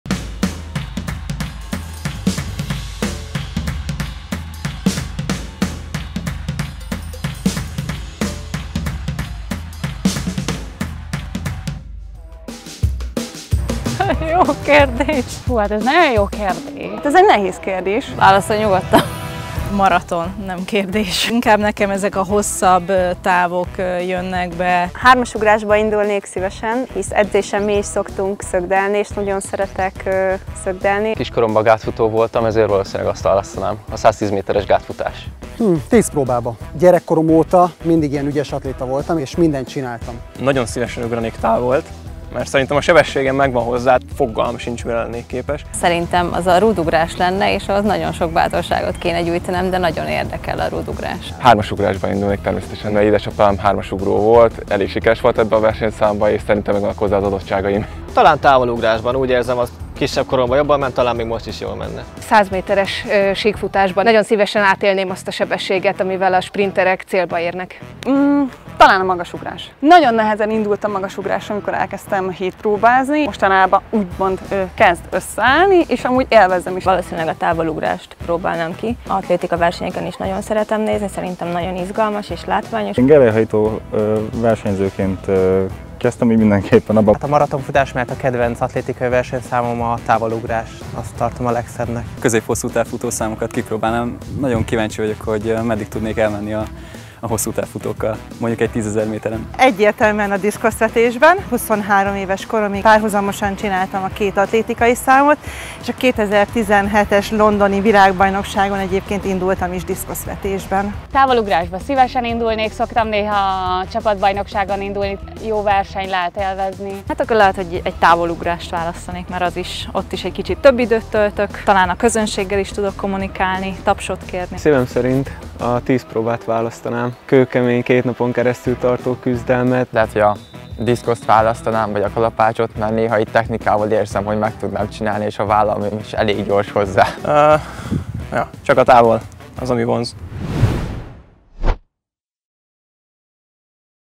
Versenyszám - Kérdezz-felelek - M4 Sport
Vajon ki, milyen versenyszámot választana magának a budapesti vb-re, hanem a sajátjában indulna? Nézzük meg együtt.